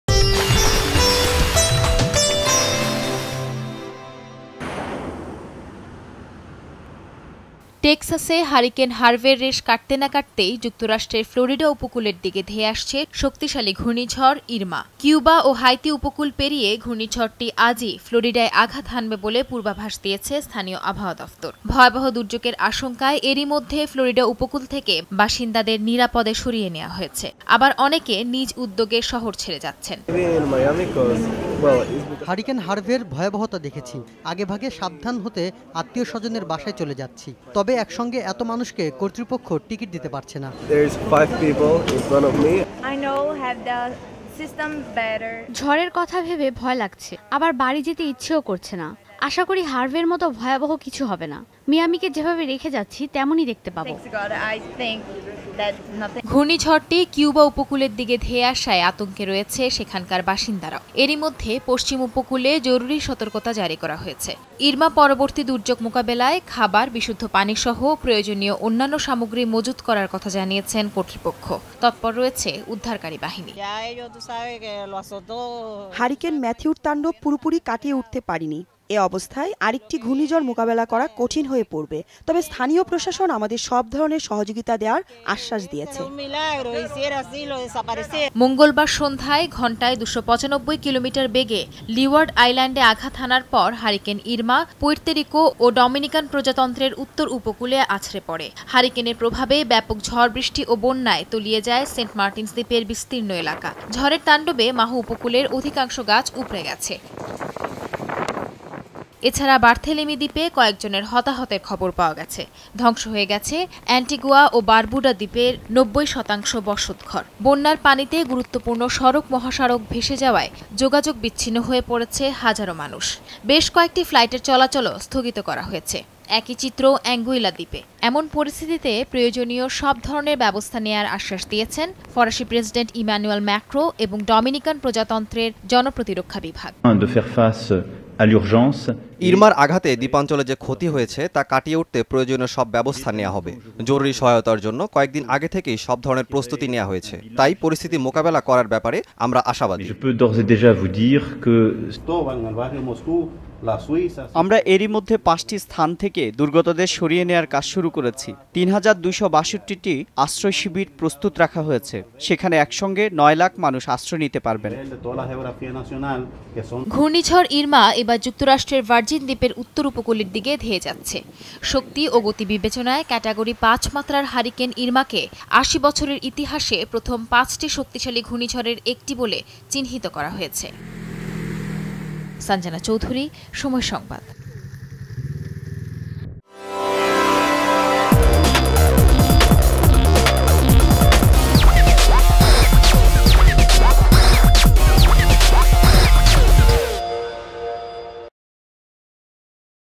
আজই ফ্লোরিডায় আঘাত হানতে পারে হারিকেন ইরমা
প্রশান্ত মহাসাগরের ক্যারিবীয় অঞ্চলে আঘাত হানার পর হারিকেন ইরমা এবার ভার্জিন দ্বীপের উত্তর উপকূলের দিকে ধেয়ে যাচ্ছে। ইরমার আঘাতে সেইন্ট বার্থেলেমি দ্বীপের অন্তত ২ জনের মৃত্যুর খবর পাওয়া গেছে। আহত হয়েছে আরো ২জন। এন্টিগুয়া ও বারবুডা দ্বীপের ৯০ শতাংশ বসতঘর ধ্বংস হয়ে গেছে বলে জানিয়েছে সেখানকার কর্তৃপক্ষ। হারিকেনের প্রভাবে ব্যাপক ঝড়-বৃষ্টি ও বন্যায় তলিয়ে গেছে সেন্ট মার্টিনস দ্বীপের বিস্তীর্ণ এলাকা।